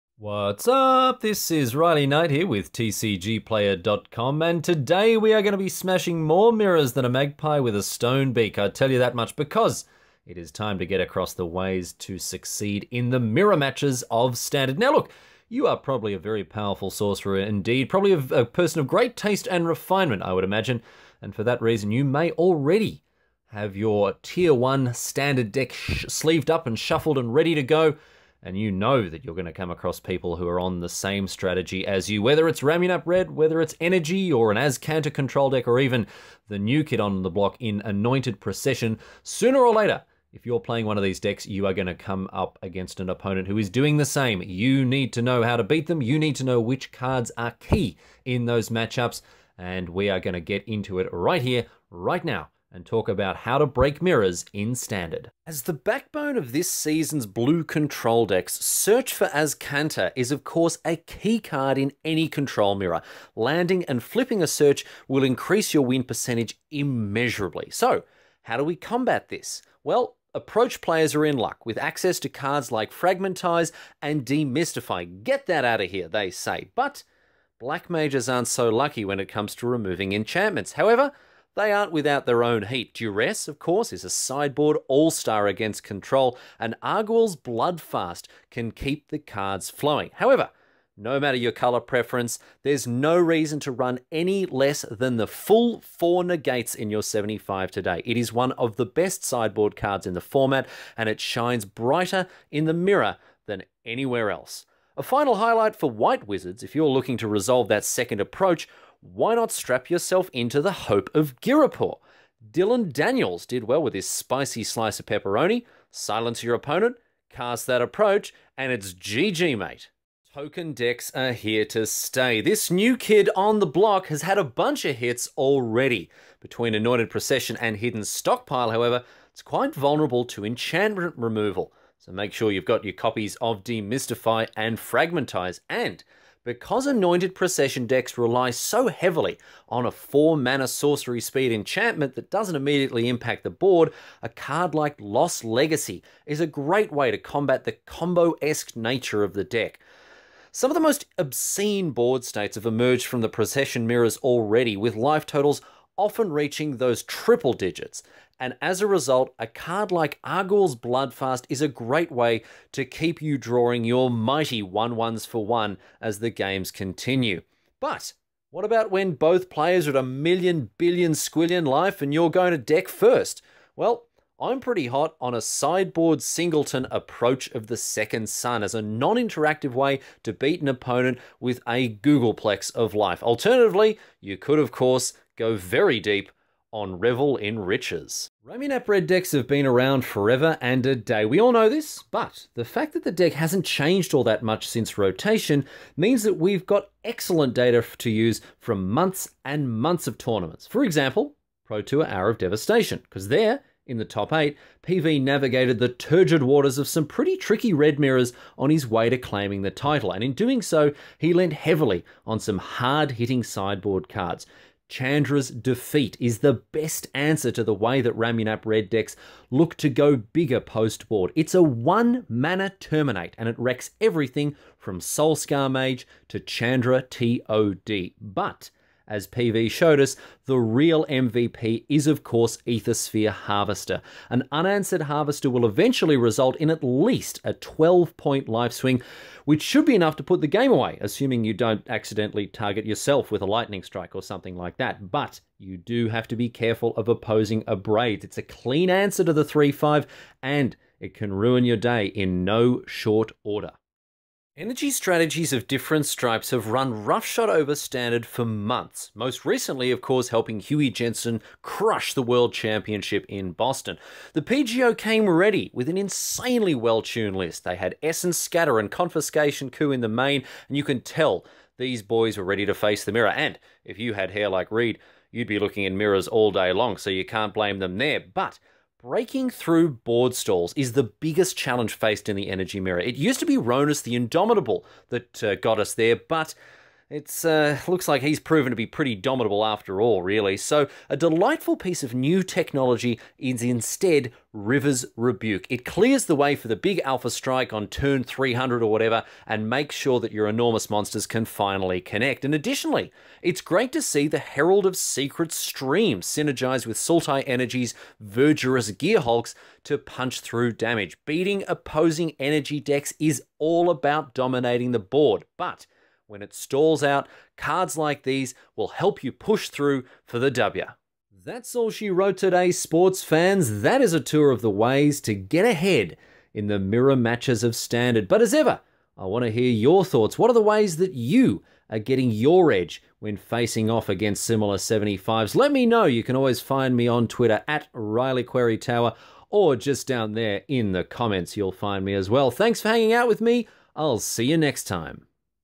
[MTG] Breaking Mirrors in Standard | Riley Knight
If you're playing one of Standard's best decks, odds are someone will be on the same wavelength as you. You need to be prepared for mirror matches. That's where Pro Magic Commentator Riley Knight comes in.